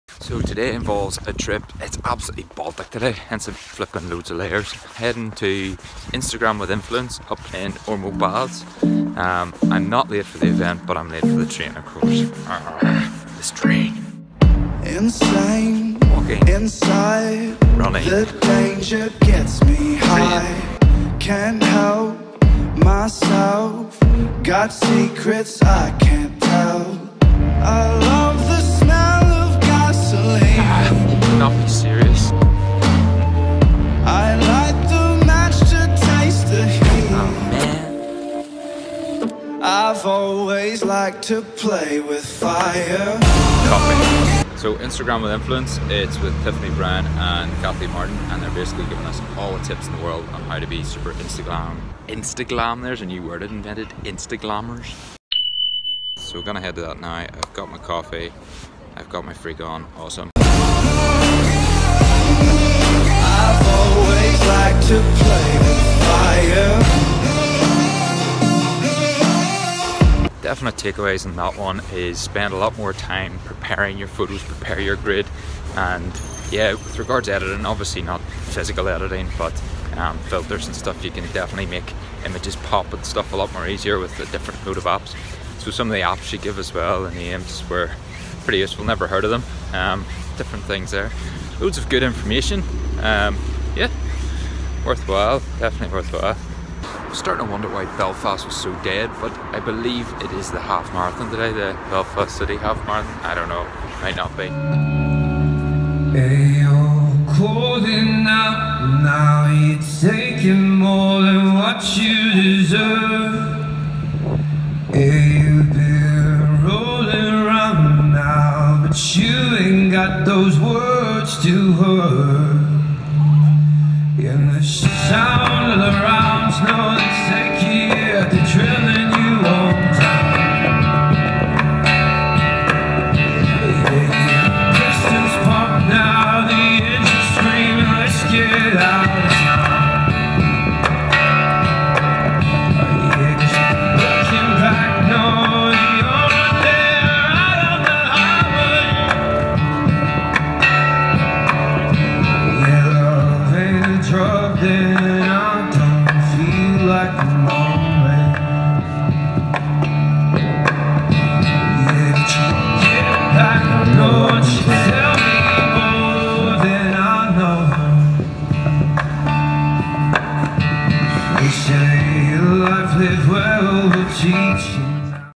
Tips for the Gram & the GREATEST busker
The day started off with a course in Instagram and how to produce a sexier account, followed by some much needed food. We then stumbled apon what I would say is the GREATEST busker I have heard in a very long time! this track recorded by someone else really shows his talent!!

Camera used
Tripod used

Workouts uploaded weekly. Drop me a comment is you want to see any specific type of workout